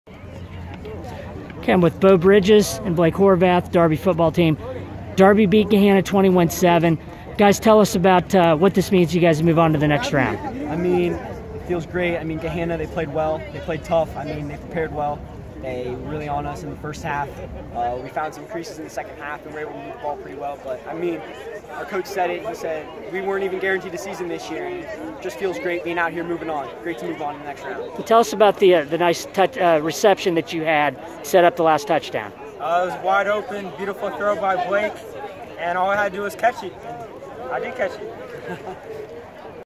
Hilliard Darby's Beau Bridges and Blake Horvath
Hilliard Darby's Beau Bridges (left) and Blake Horvath discuss their team's 21-7 win Oct. 9 at Gahanna in the first round of the Division I, Region 3 playoffs. [Jarrod Ulrey/ThisWeek]